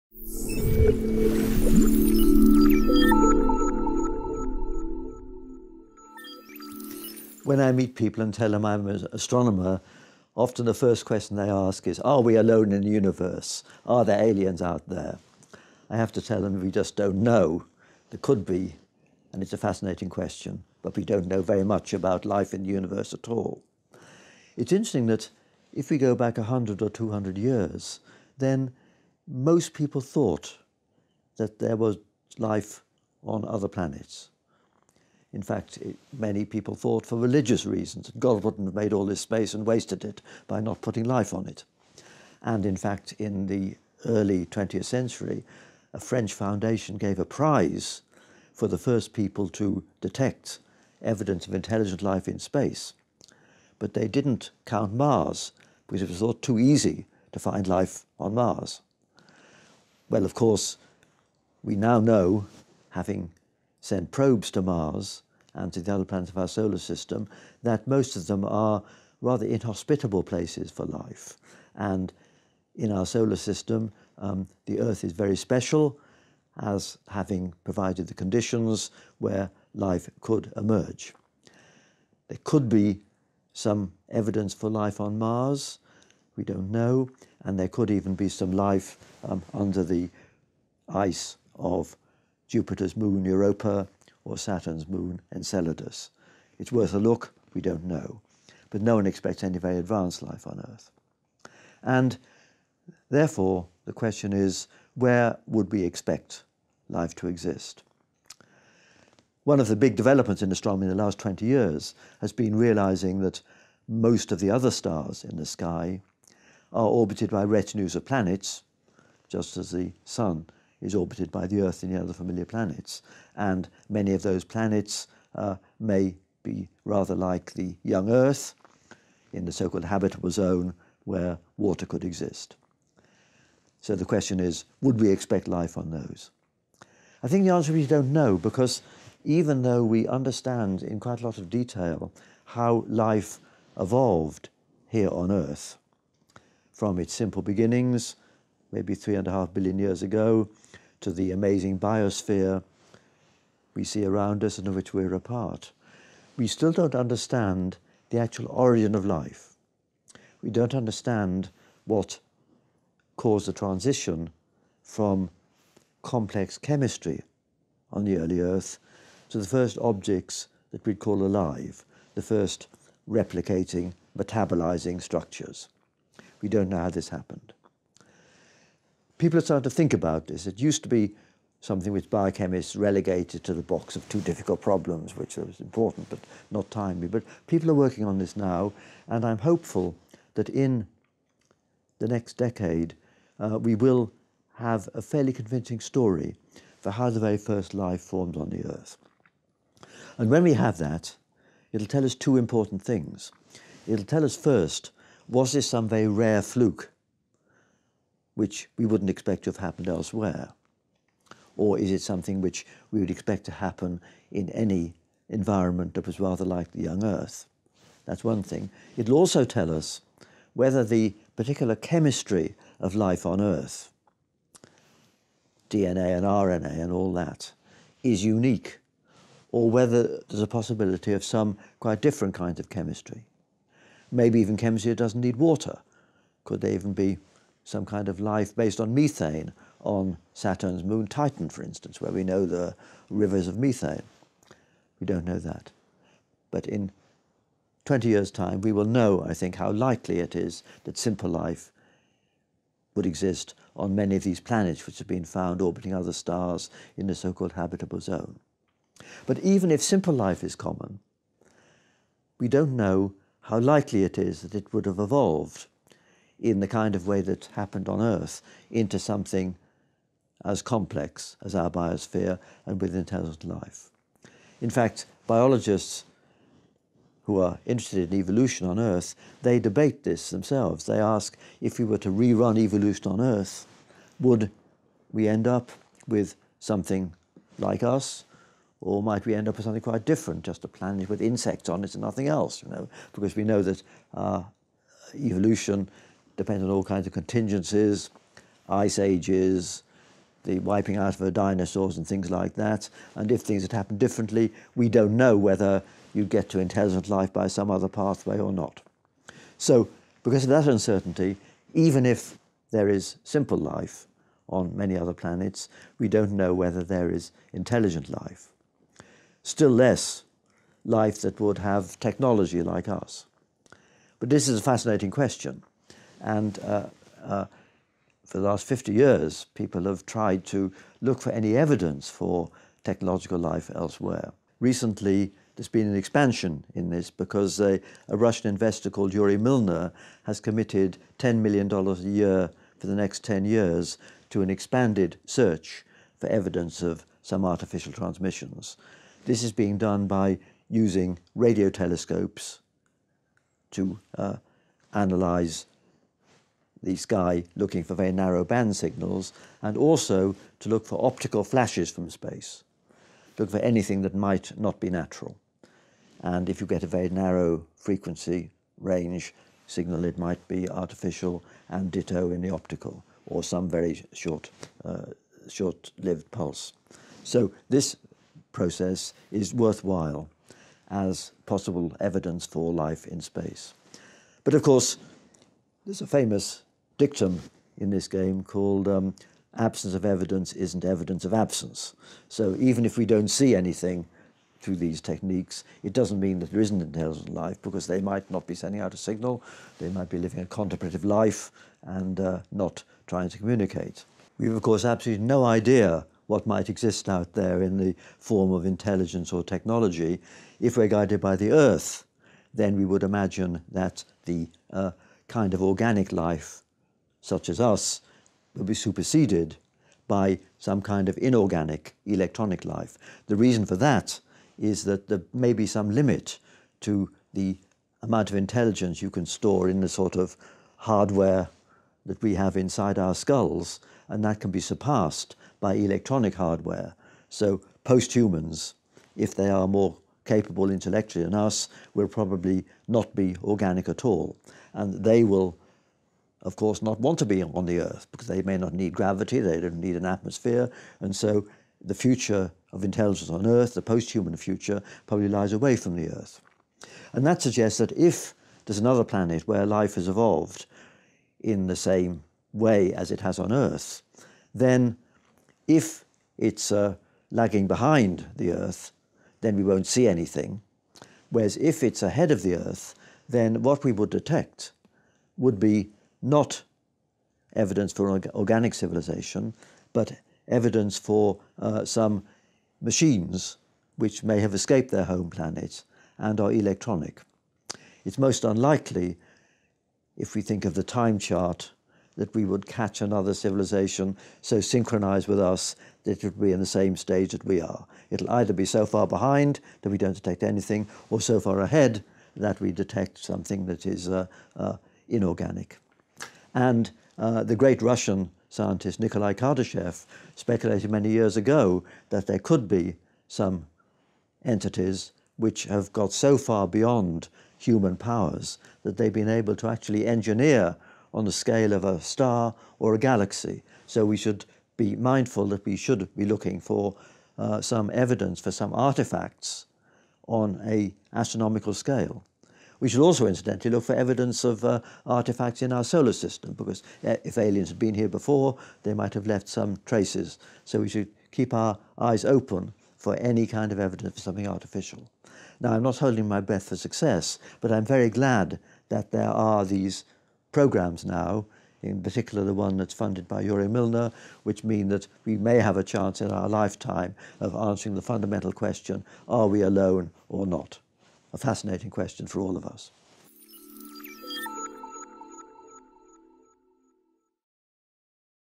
Life in Space — Martin Rees / Serious Science
Cosmologist Martin Rees on life in the Solar System, artificial transmissions, and inorganic civilizations.